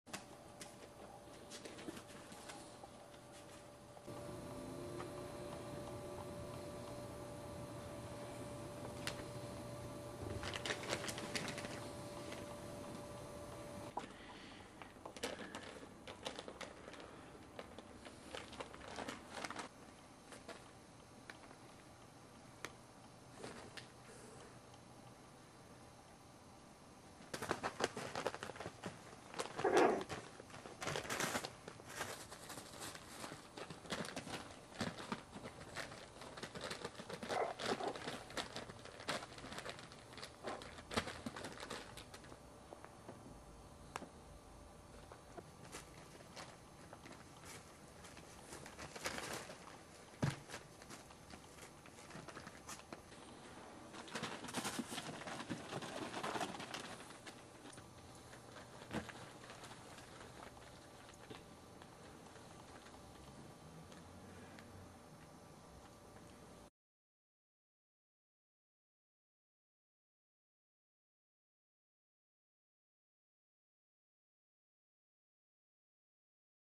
Chihuahua Puppies and William (in HD)
Three Chihuahua puppies react to our Maltese dog William .